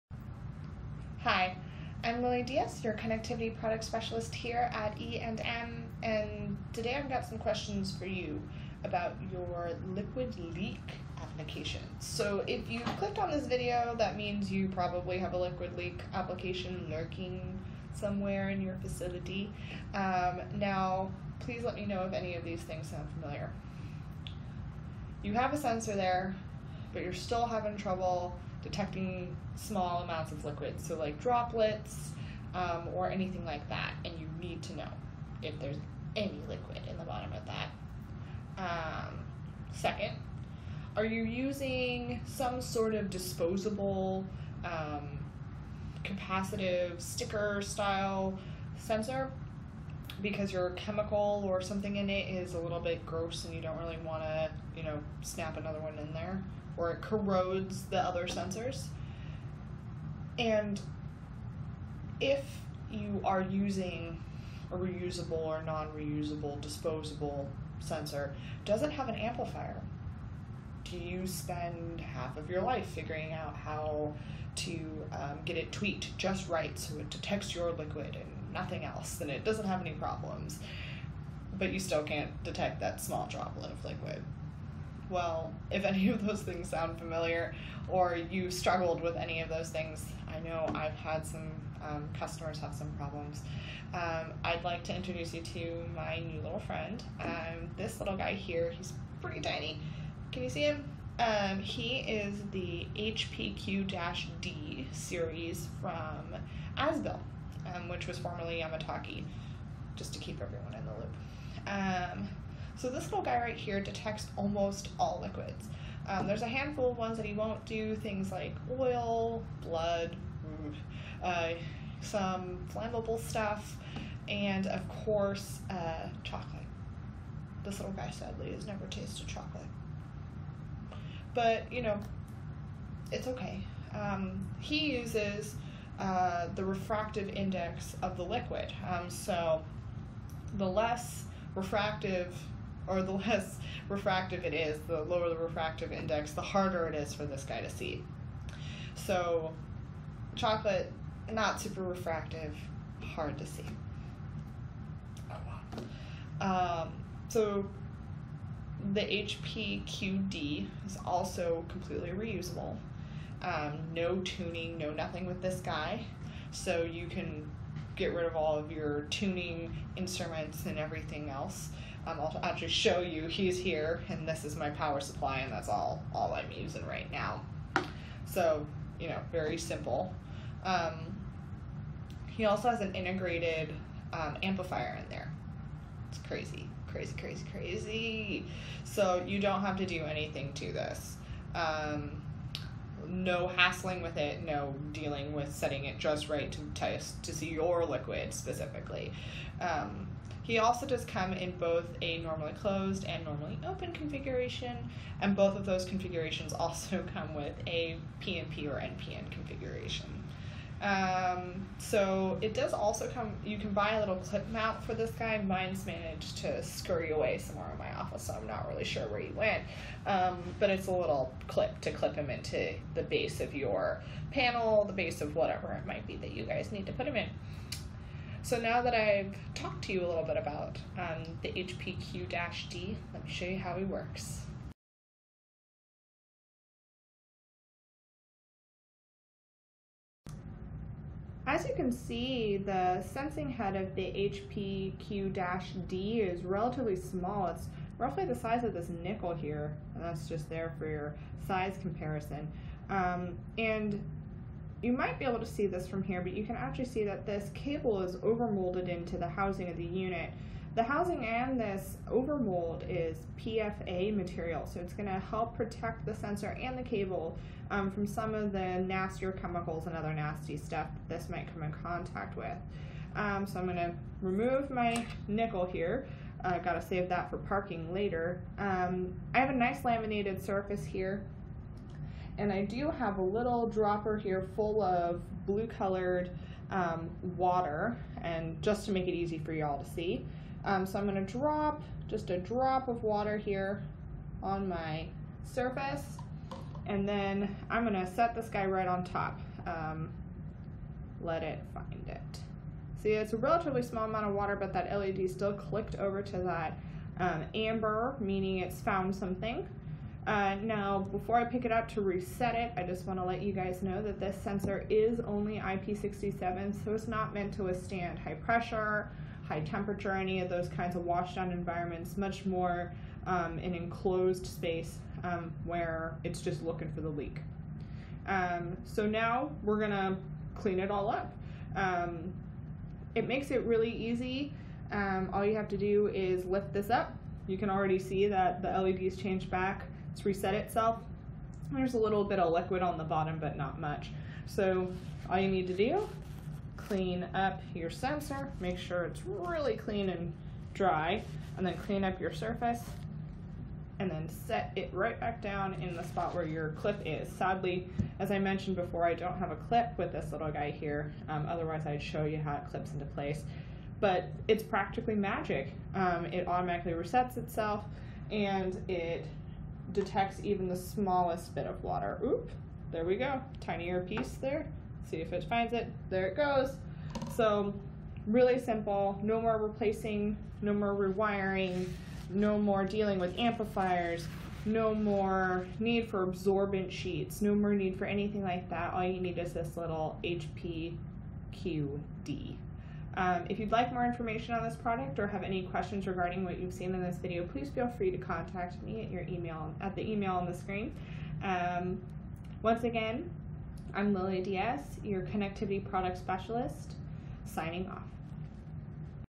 Reusable, Hassle-Free Liquid Leak Detector | EandM
Are you currently using adhesive paper thin capacitive sensors or another disposable method to detect leaks?

Are you having trouble detecting small amounts of liquid with your current solution?

Are you spending half your life fiddling with the external amp?

If so, watch this video and learn about the HPQ-D Series Liquid Leak Detector from azbil, formally Yamatake.